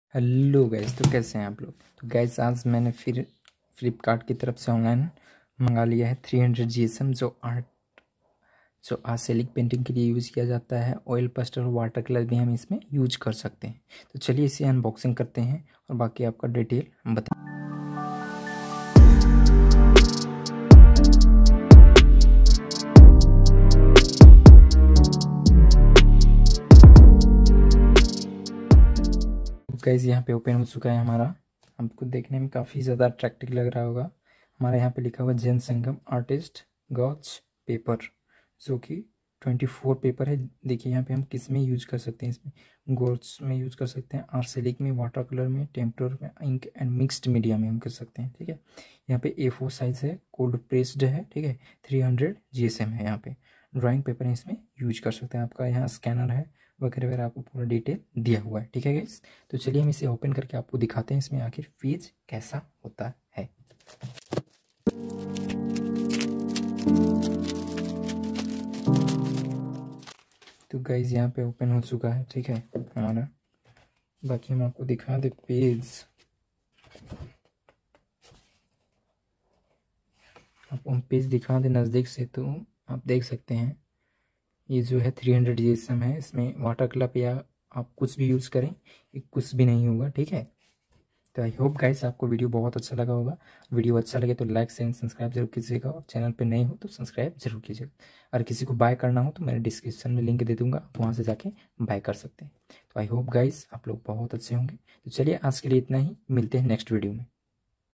Unboxing ZEN SANGAM 300gsm paper for Artist | drawing paper unboxing 2022 new #61
Unboxing ZEN SANGAM 300gsm paper  for Artist | drawing paper unboxing 2022 new

@neerajarts111 @Vlogging life Neeraj @ArtArena @ArtistShikhaSharma @FarjanaDrawingAcademy @Aryanvermastudios

Hey Guy's  I"M Neeraj welcome to my art chanel

☑️My Camel oil pastel

☑️My 2nd phone

Love u My all sisters and brothers 🥰🥰🥰🥰🥰

Tags-

Hastags-

Jharkhand (chouparan)